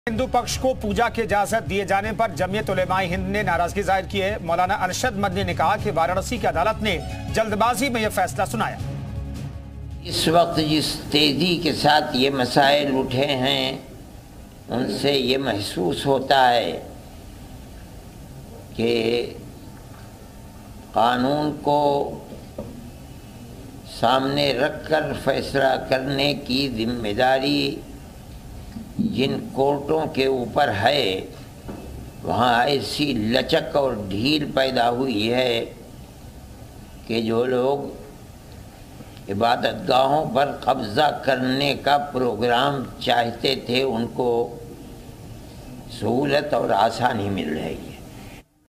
Varanasi: ज्ञानवापी पर गदर जारी, व्यास तहखाने में जारी रहेगी पूजा| Gyanvapi Case
Varanasi: ज्ञानवापी पर गदर जारी, व्यास तहखाने में जारी रहेगी पूजा, 6 फरवरी को मामले में अगली सुनवाई